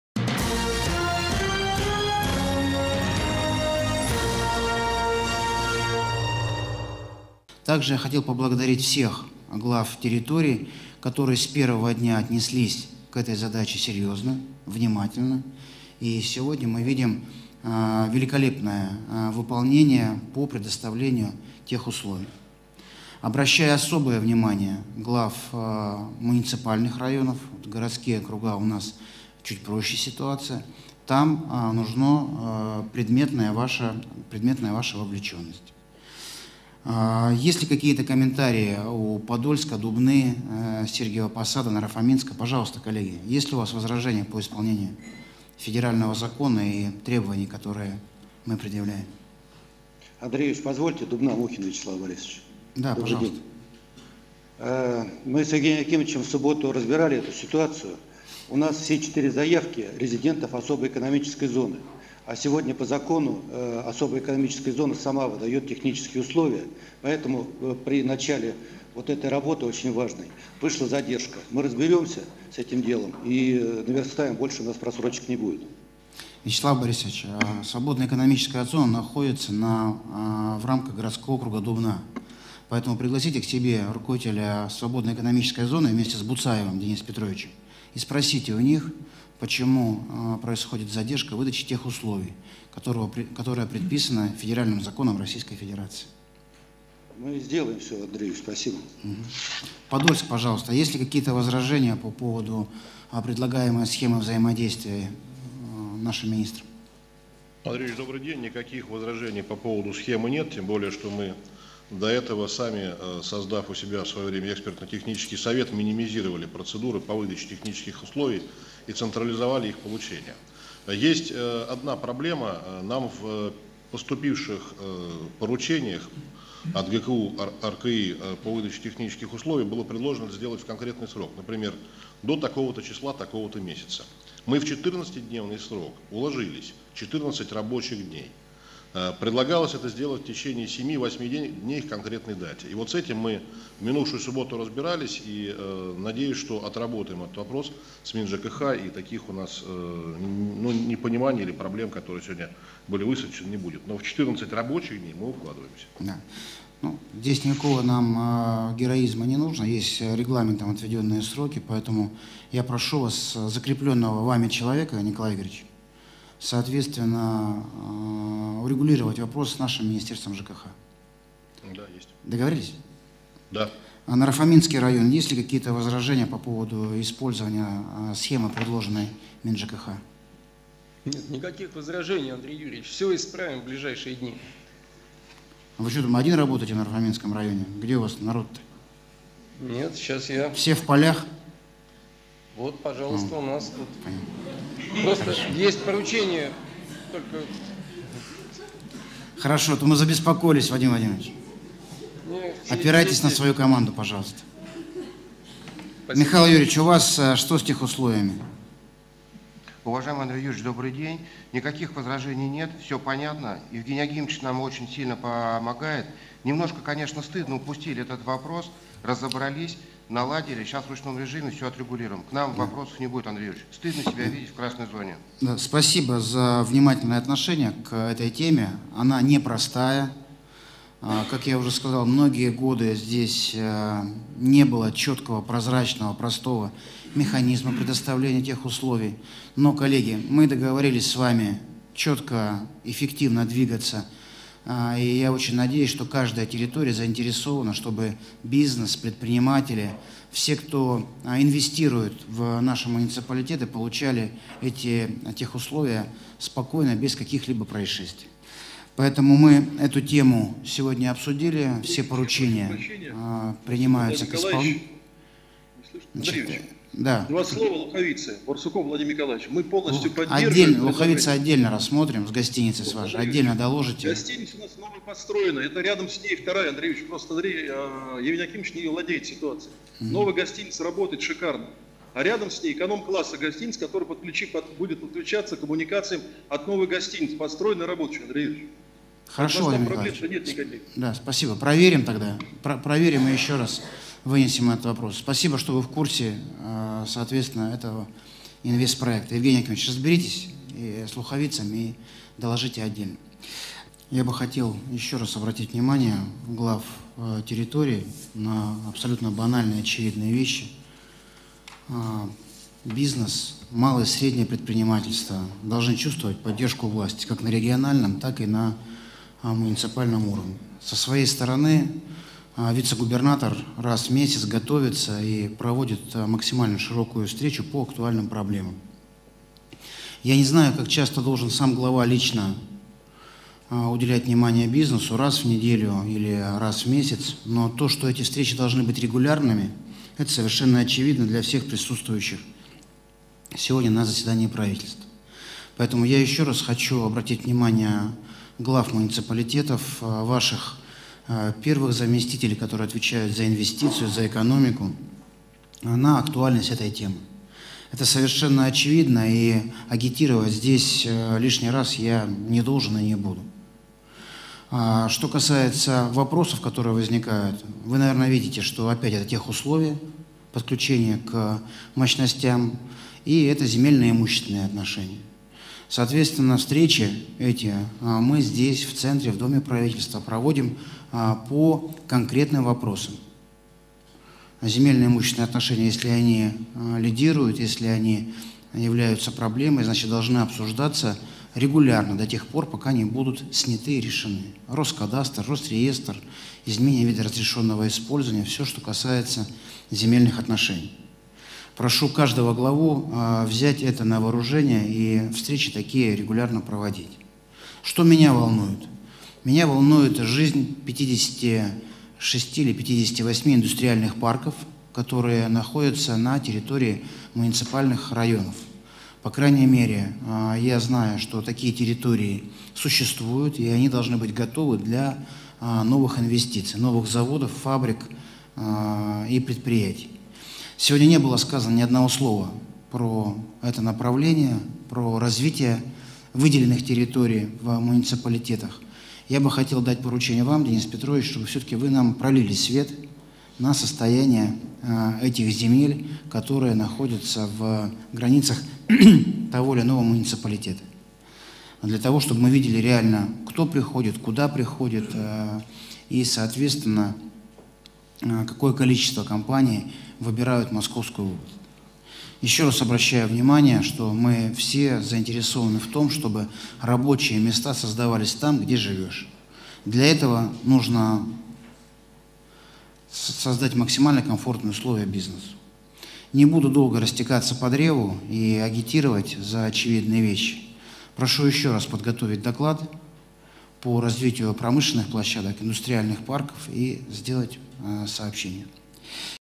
МО должна стать самым гостеприимным местом для бизнеса - губернатор
Московская область должна стать самым гостеприимным местом для развития бизнеса, заявил губернатор Андрей Воробьев.